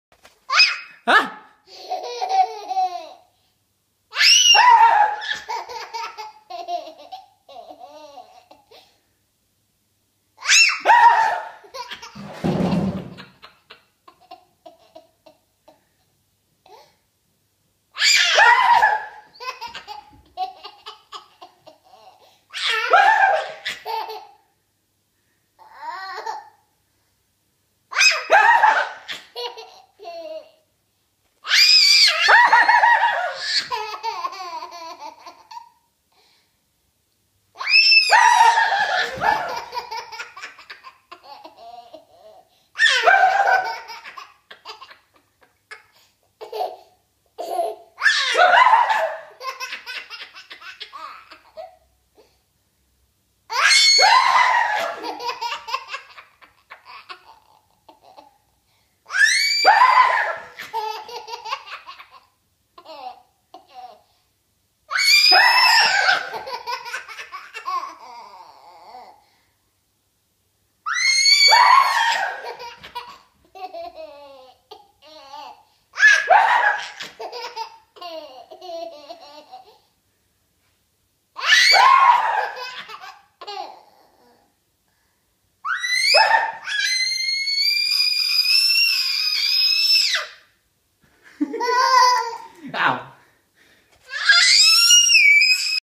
Laughter is contagious
Playing with my little one...she screams, I act scared (and scream like a girl), and she laughs historically.  It brings warmth to my heart and hope it does yours as well.